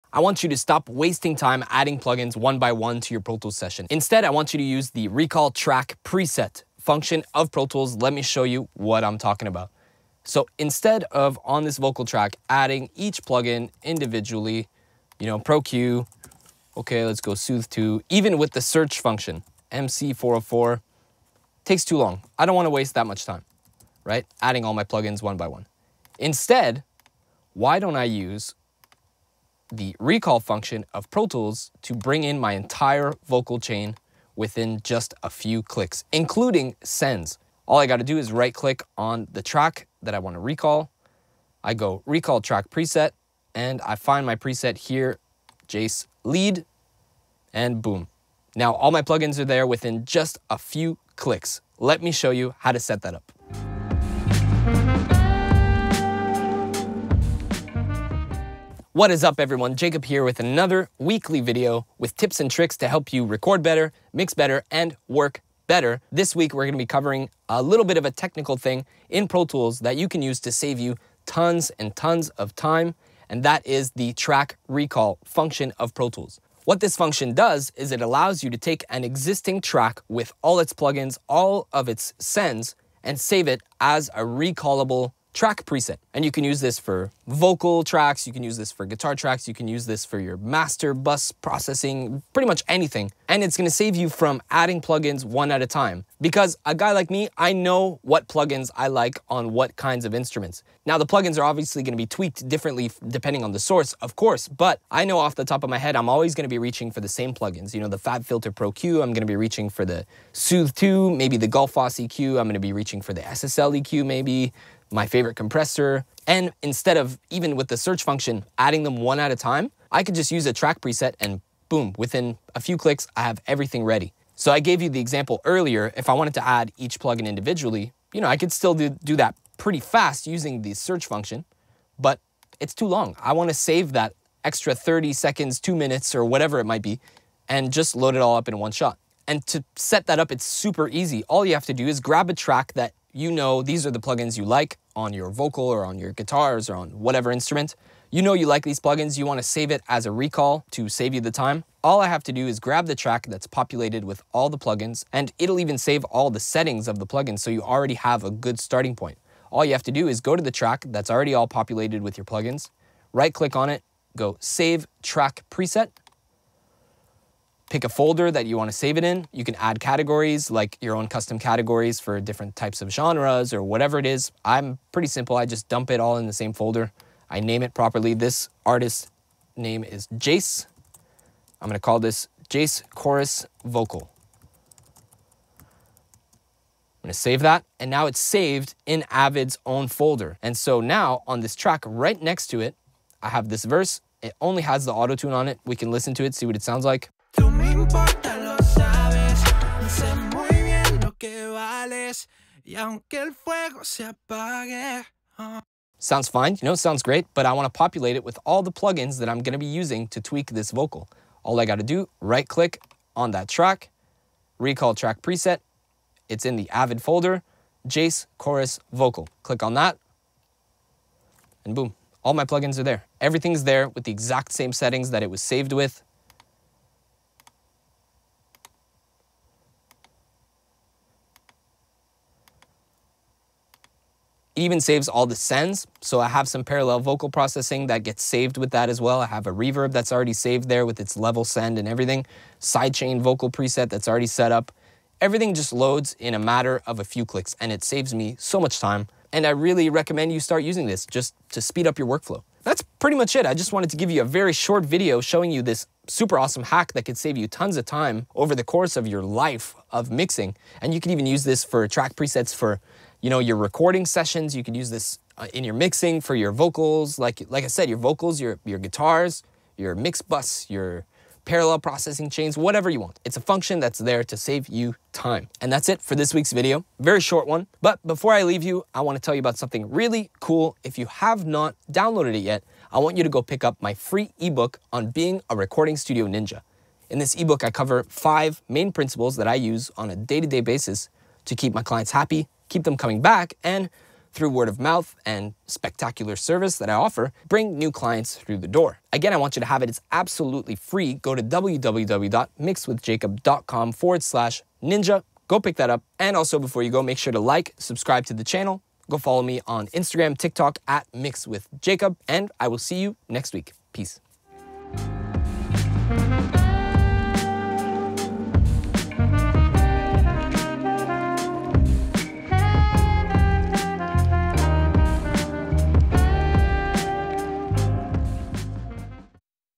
STOP WASTING TIME! Use TRACK PRESETS in Pro Tools 🔥
CUSTOM TRACK PRESETS IN SECONDS 🤯

Hey, you!

Welcome back to this week's tips and tricks video!

Today, a VERY UNDERRATED and SUPER HELPFUL workflow trick in Avid Pro Tools.

The Track Preset and Recall function in Pro Tools is very underutilized as far as I can tell in my immediate surroundings. You can easily save yourself minutes EVERY SINGLE TIME you load up a new track in your mixing, mastering, or recording sessions.

Create custom presets with the plugins that you KNOW and LOVE so that you can INSTANTLY recall them in just a few clicks 🤩

Song: Unreleased by @jacecarrillo